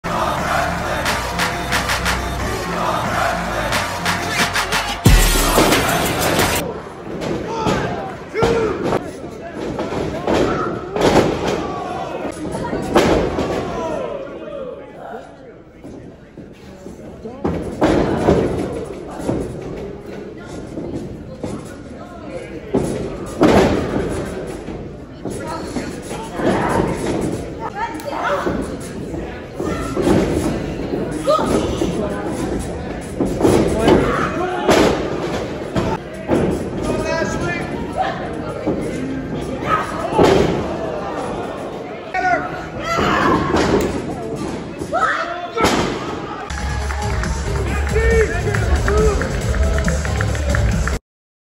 Ashley D’Amboise vs Layna Lennox : Women’s Wrestling Army : WLW Views
Ashley D’Amboise vs Layna Lennox from Women’s Wrestling Army in Chicago in July.